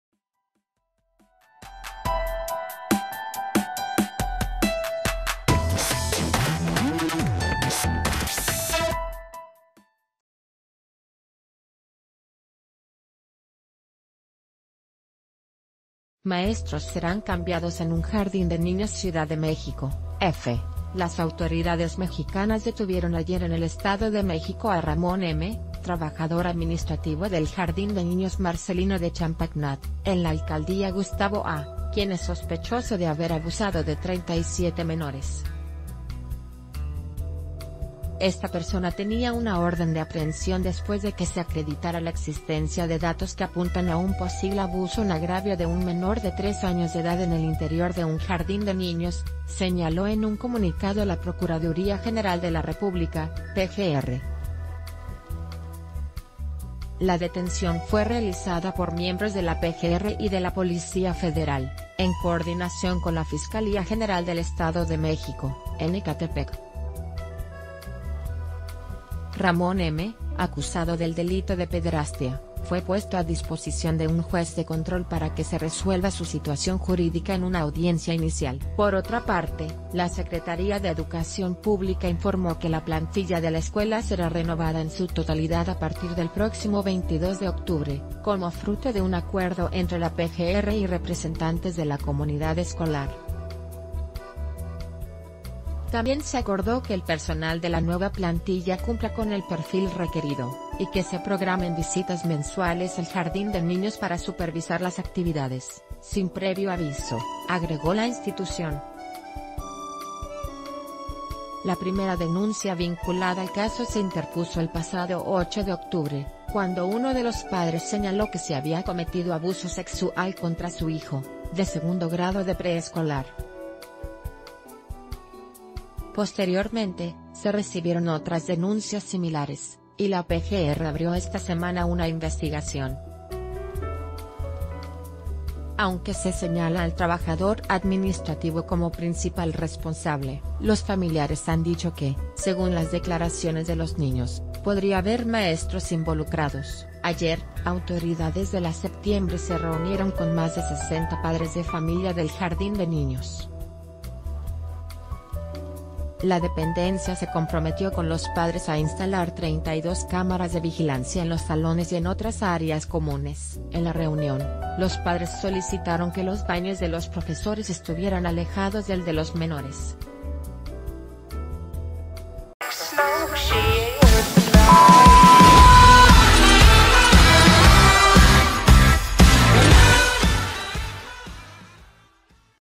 Autoridades del Estado de México capturan a un violador
Maestros serán cambiados en un jardín de niños CIUDAD DE MÉXICO (EFE).— Las autoridades mexicanas detuvieron ayer en el Estado de México a Ramón “M”, trabajador administrativo del jardín de niños “Marcelino de Champagnat”, en la alcaldía Gustavo A, quien es sospechoso de haber abusado de 37 menores.